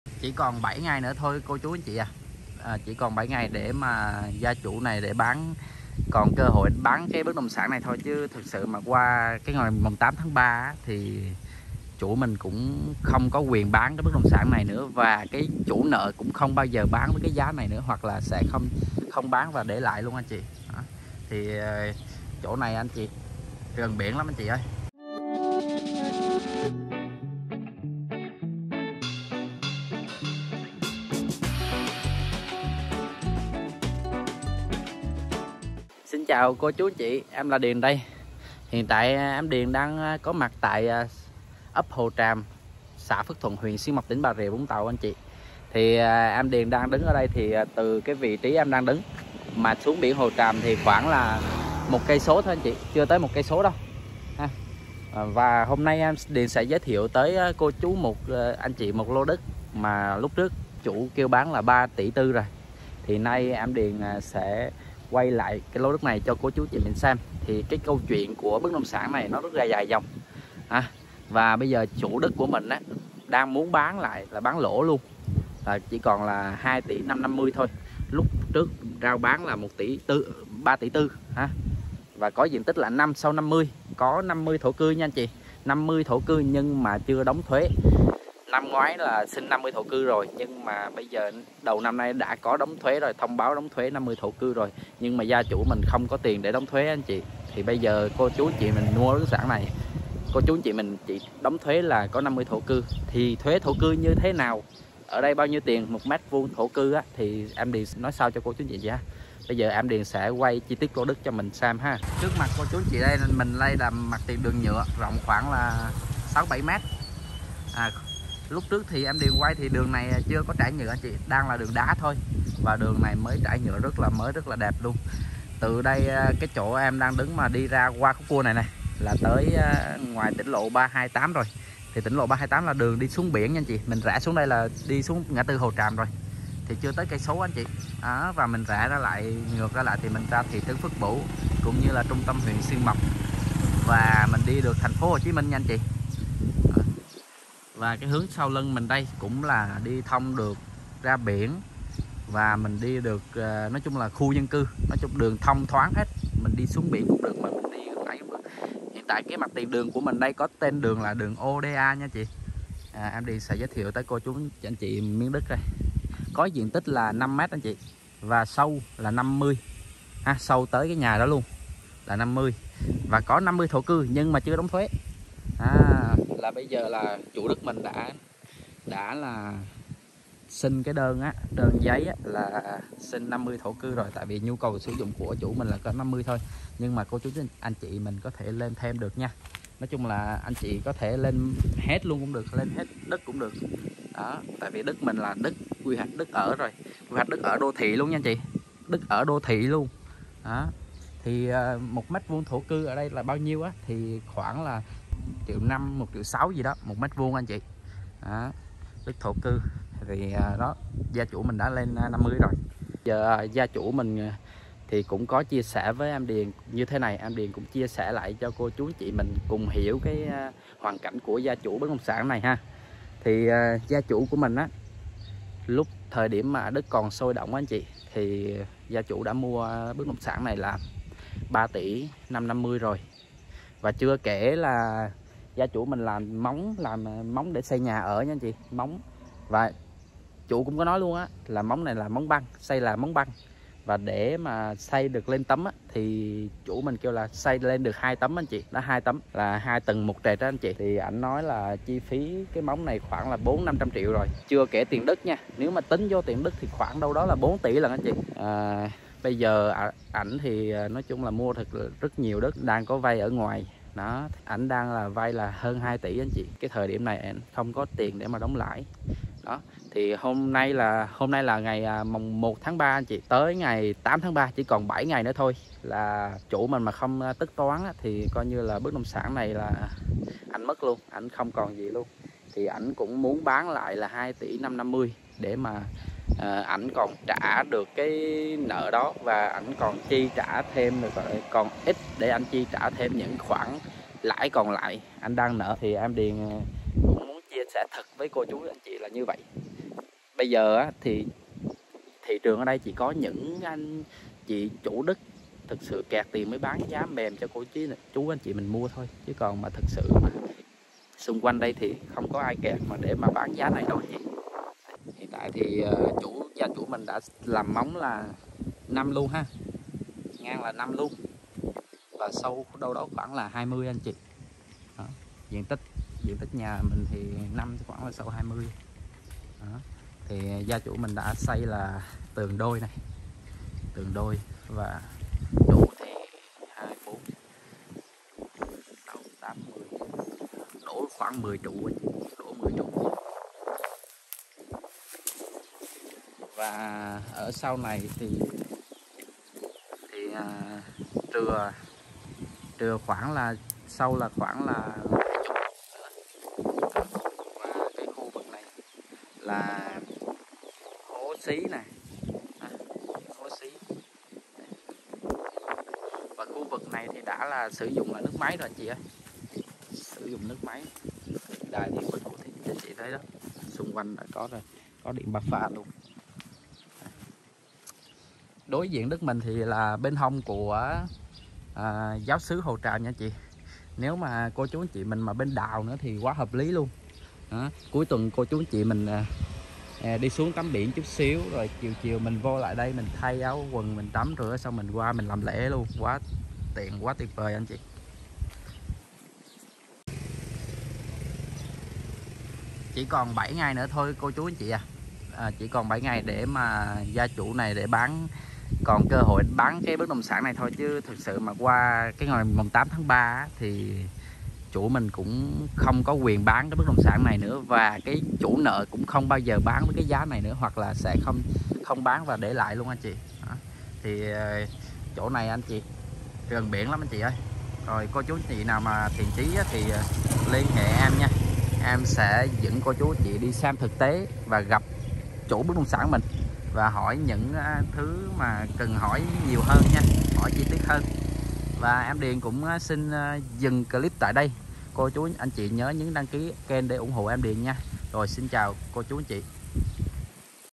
Cách BIỂN HỒ TRÀM 1KM đại hạ giá từ 3 tỷ 550 còn 2 tỷ 550 | 5x50 50 thổ cư | LH 0788668834 (Điền)
👉Vị trí ngay BIỂN HỒ TRÀM . Đầy Đủ Mọi Tiện Ích: Uỷ Ban, Trung tâm hành chính, Chợ, trường học, siêu thị, trung tâm vui chơi...
* Tọa lạc : xã Phước Thuận , Huyện Xuyên Mộc , Tỉnh BRVT
👉Hạ tầng đường nhựa , đèn đường, Điện ,Nước , ...
 👉Diện tích : 5x50
👉 Thổ cư : 50m
🏁Giá chỉ : 2 tỷ 550 công chứng liền
☎️Gọi ngay: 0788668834 ZALO Điền
🌈 Đặc biệt tại đây có rất nhiều khu du lịch,Dự án,Resort nổi tiếng như: Biển Hồ Tràm, suối nước nóng Bình Châu, Novaworl, Meliã, Cầu ngắm biển Hamptons Pier, Sanctuary Ho Tram, v.v..